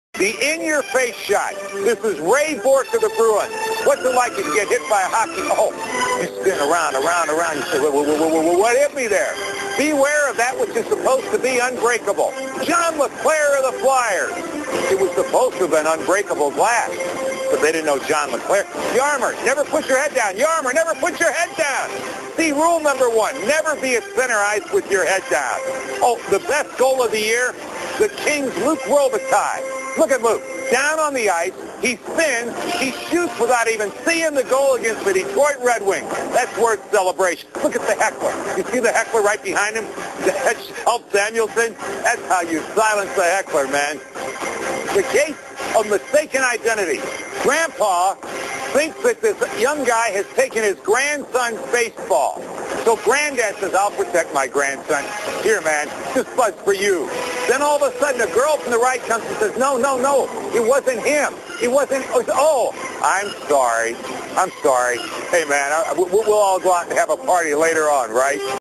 Sundin rocks Jagr and Ulf as a Wing
Also see Ulf spray water at a fan. Vindigidy